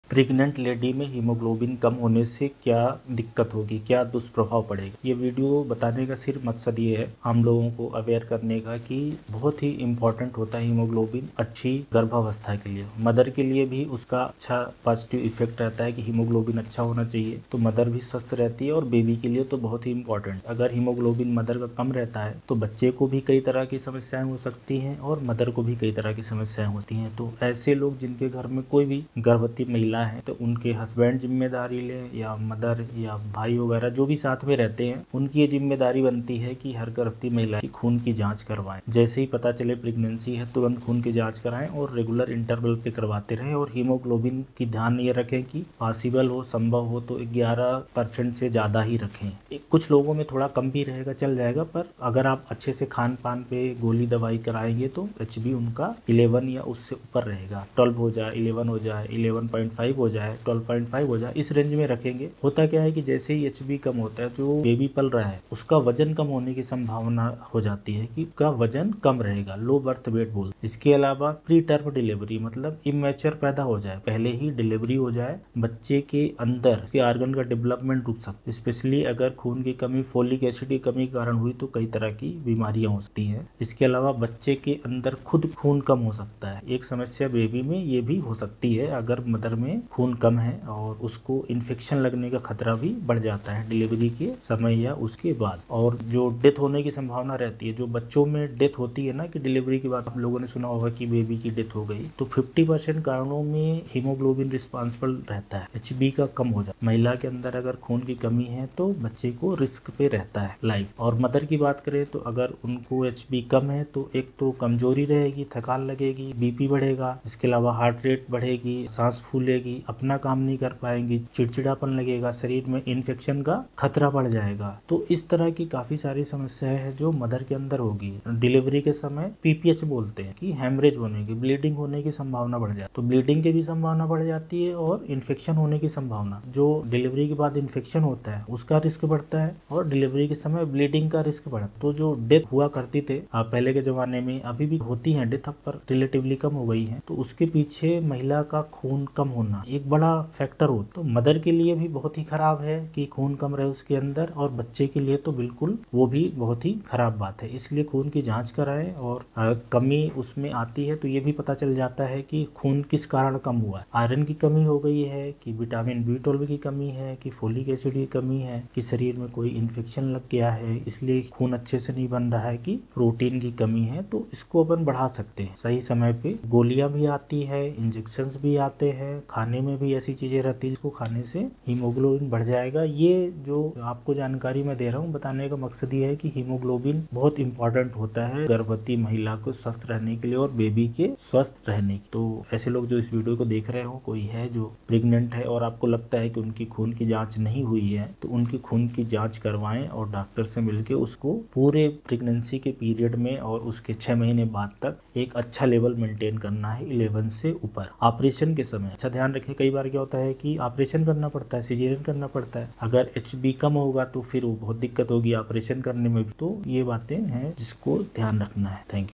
PREGNANY me Hemoglobin kam hone ke khatre | गर्भावस्था में एनीमिया के नुक़सान
Pregnancy me Low Hb hone ke kya nuksan hai mother and child ko? Iss video me bataya gya hai. Garbhawastha ke dauran kitna heemoglobin hona chahiye.

Anemia during pregnancy increases the risk of premature birth (when delivery occurs before 37 complete weeks of pregnancy). Iron deficiency anemia during pregnancy is also associated with having a low birth weight baby and postpartum depression.